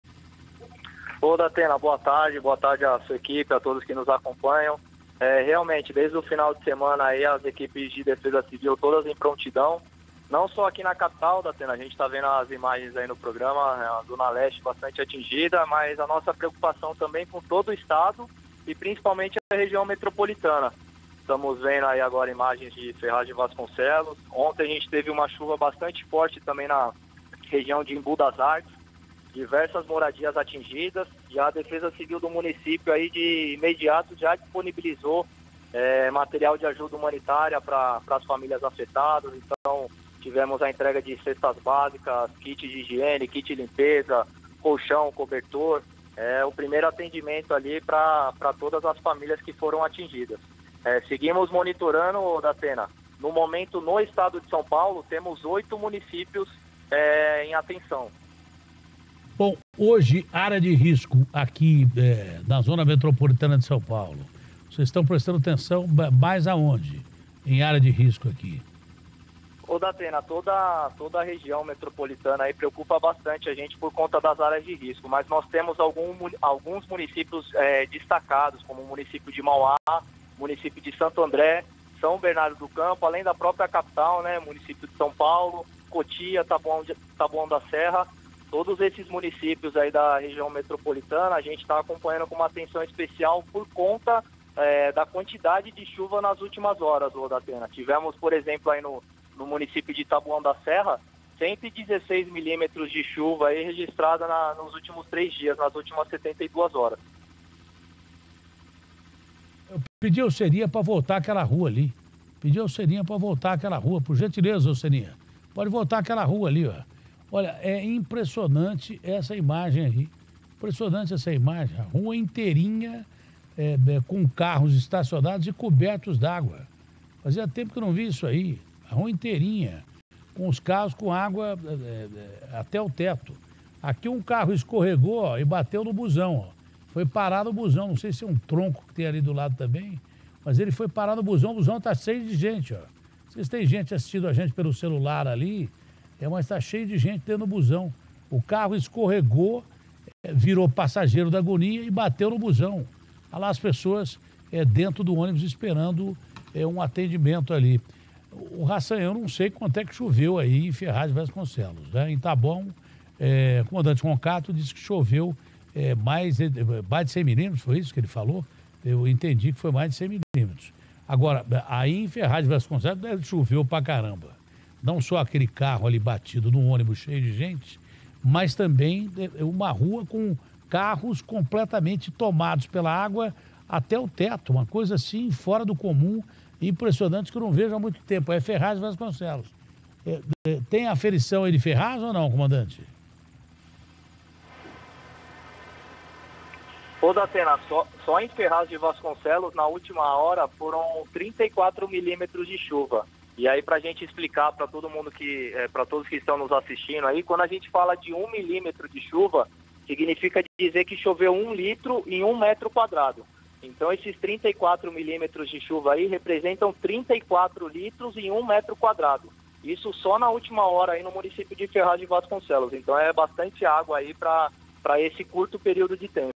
Mais de 500 árvores caíram em São Paulo
No estado de SP, oito cidades estão em estado de atenção, segundo o representante da defesa civil, Tenente Roncatto.